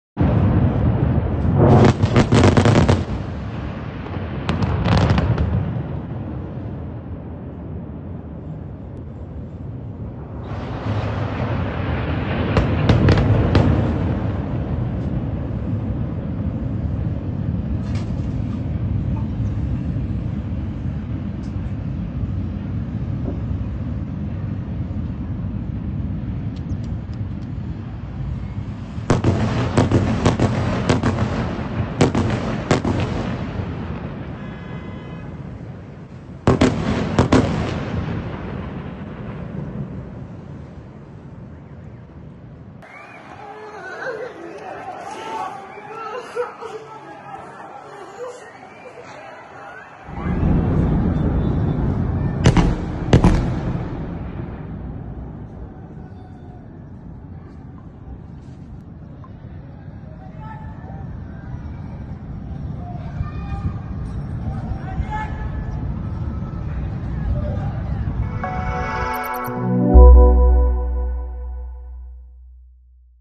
لحظة استهداف الطيران الإسرائيلي عمارة التاج 3 وعددا من المنازل المحيطة بها وسط غزة دون سابق إنذار
دمّرها على رؤوس ساكنيها.. لحظة استهداف الطيران الإسرائيلي عمارة "التاج 3" وعدداً من المنازل المحيطة بها وسط غزة دون سابق إنذار